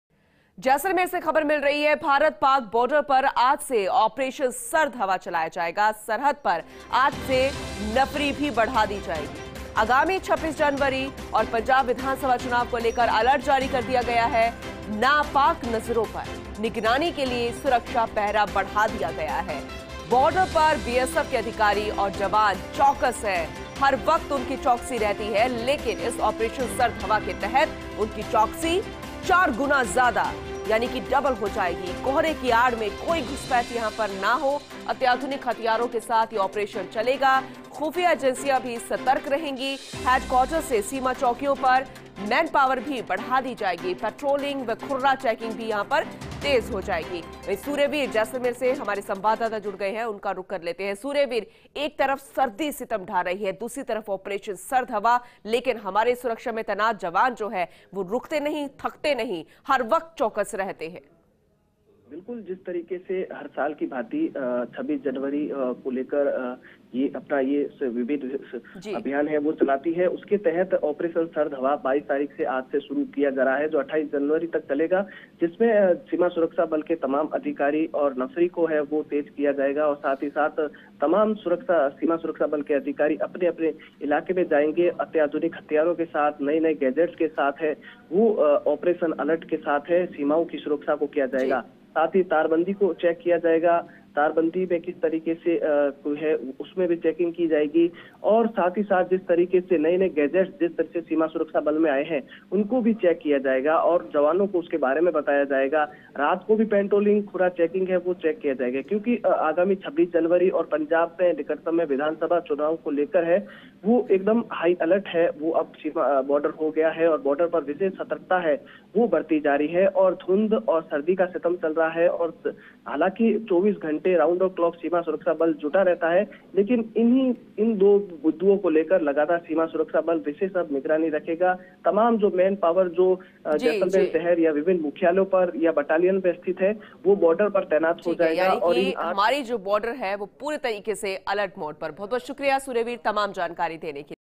भारत पाक बॉर्डर पर आज से "ऑपरेशन सर्द हवा", अत्याधुनिक हथियारों के साथ चलेगा ऑपरेशन | Jaisalmer News
About this Video:From today on the Indo-Pak border, "Operation Chid Hawa", the operation will run with state-of-the-art weapons. Jaisalmer News